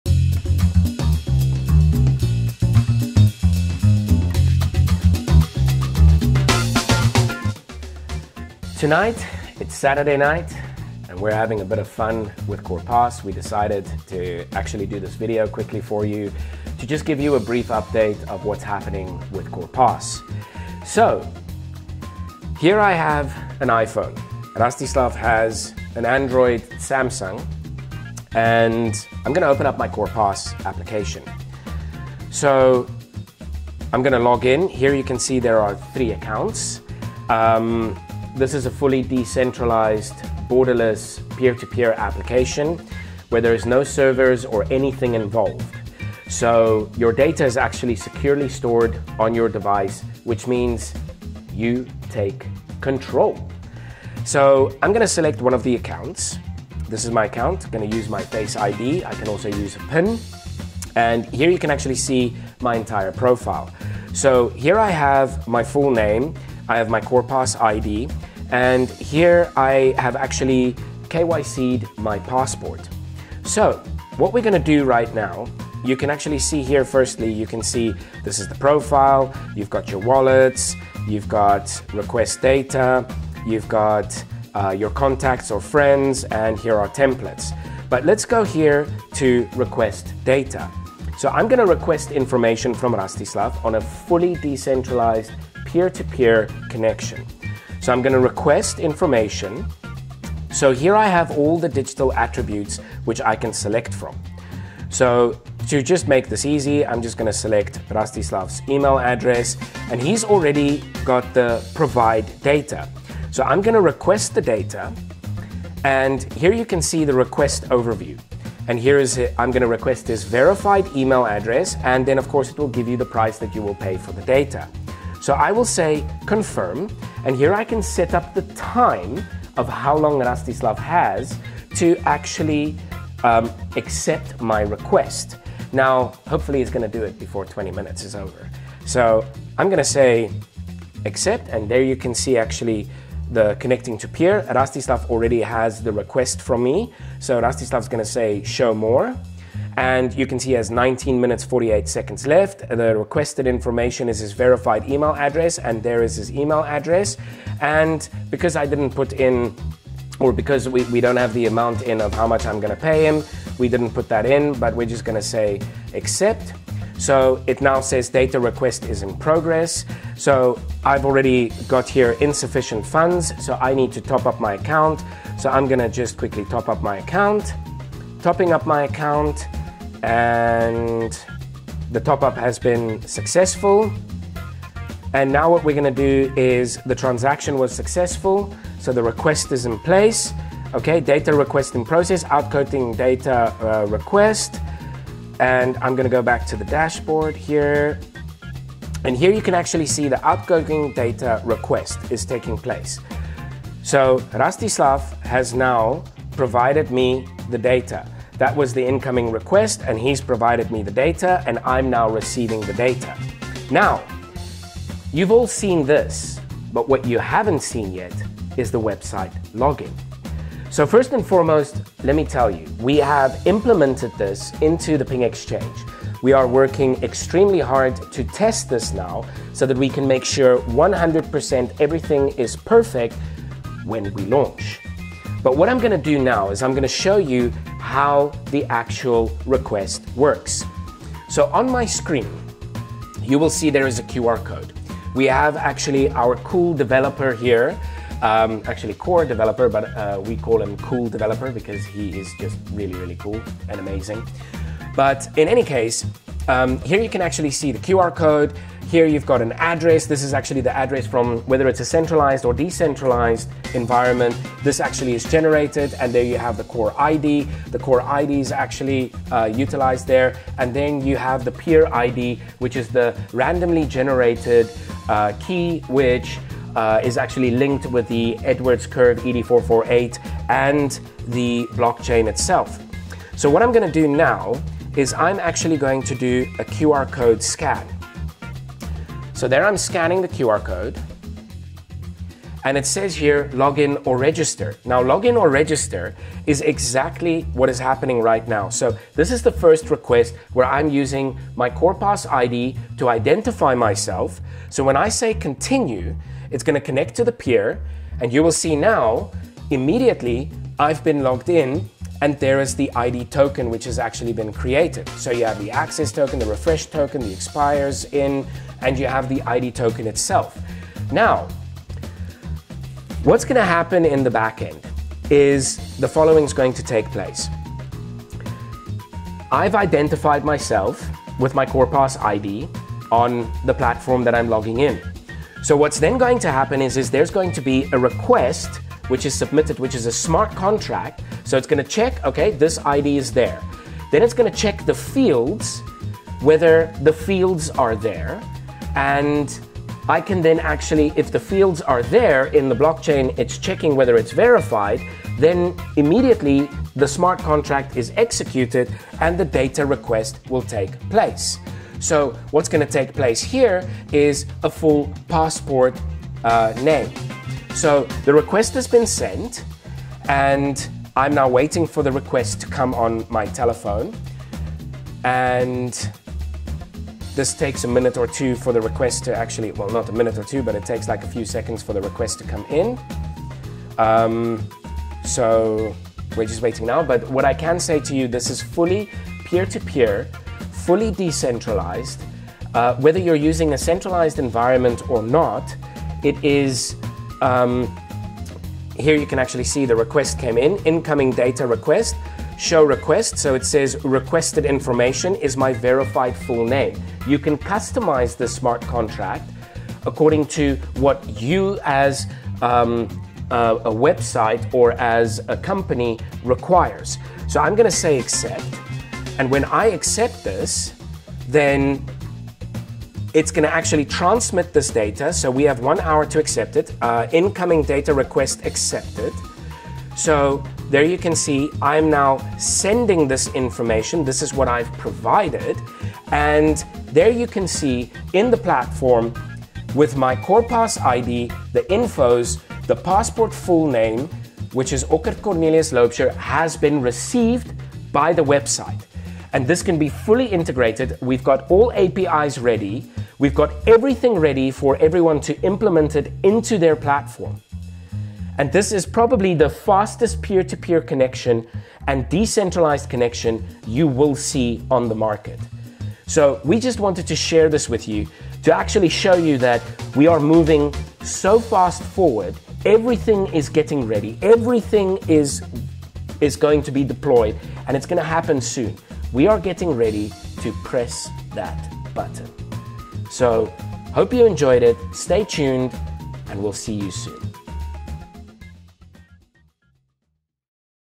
CorePass verification and login or registration
A brief overview of how the CorePass application works for peer to peer, login or registration without even the need of filling out a form.